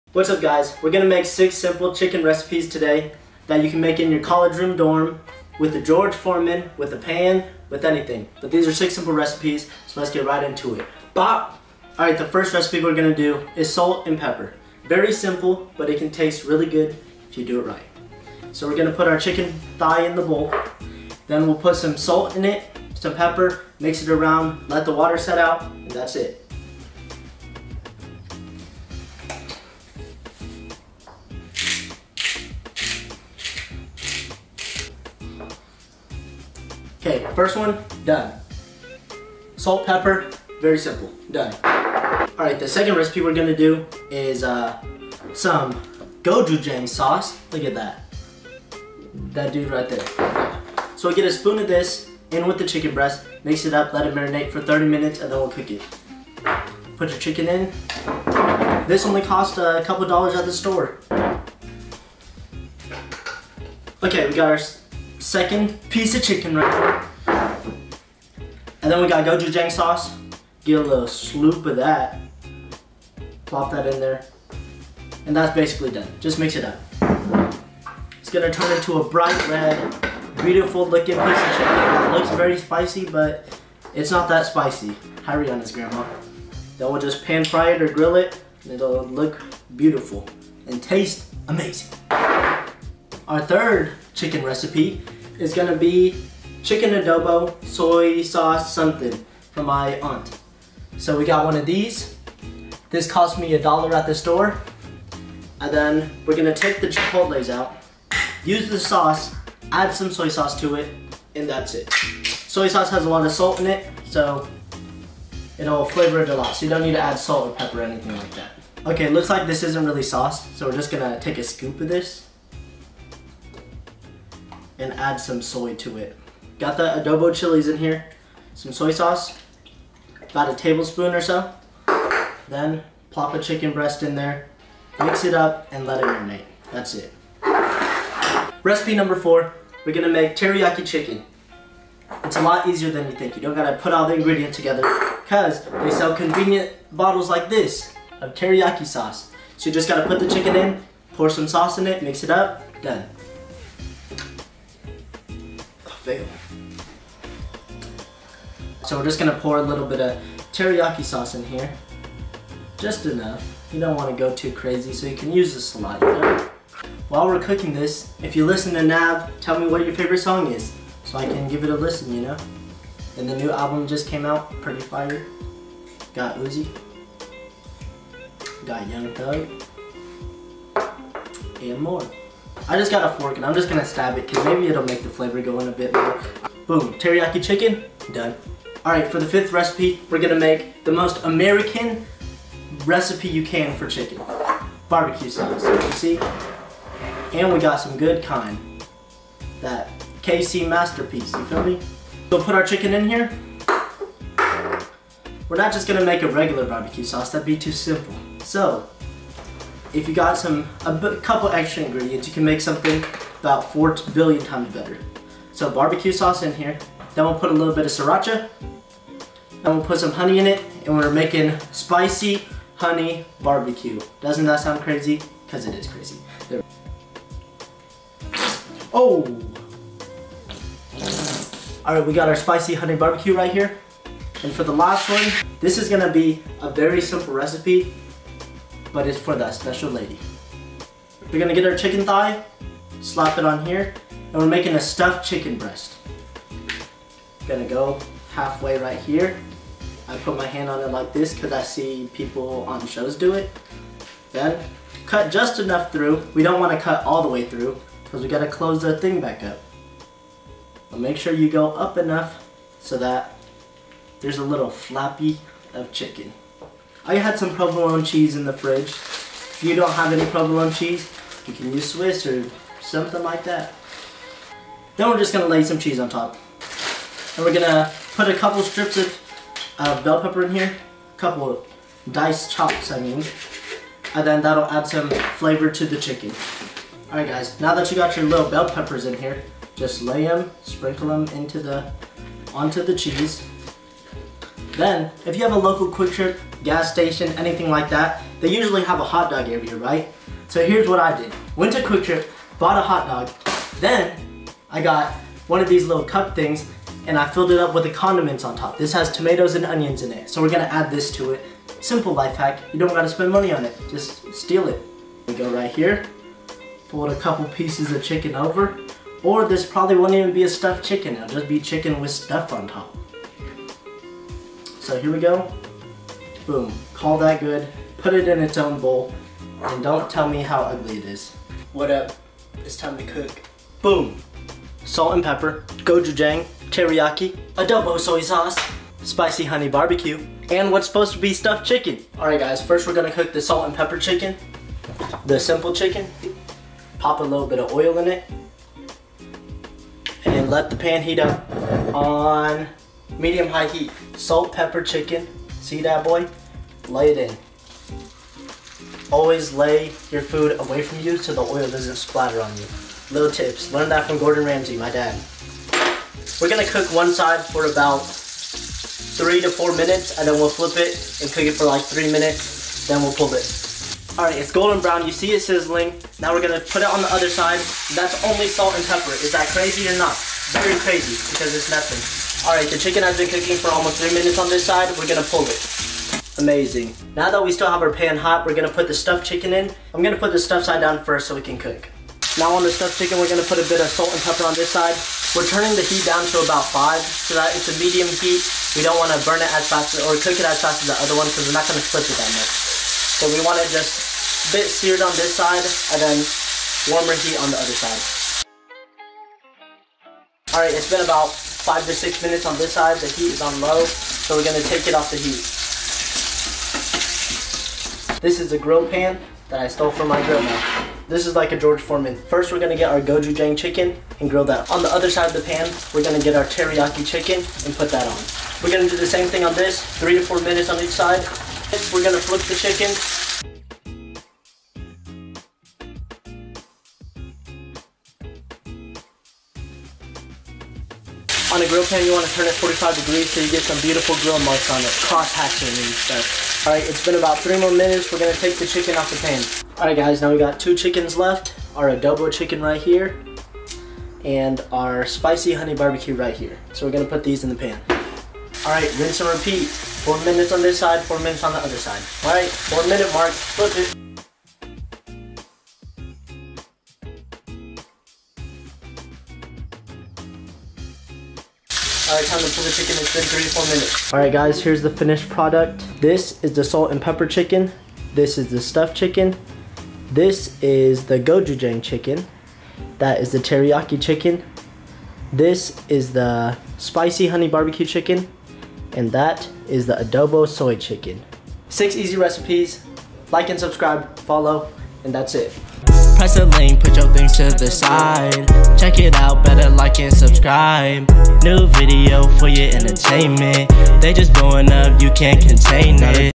6 Simple Chicken Recipes For College Dorms
Thanks for checking out the video. I understand that college students are broke and do not have time for crazy recipes. So welcome to this video where I will teach you to make easy chicken recipes fit for a dorm.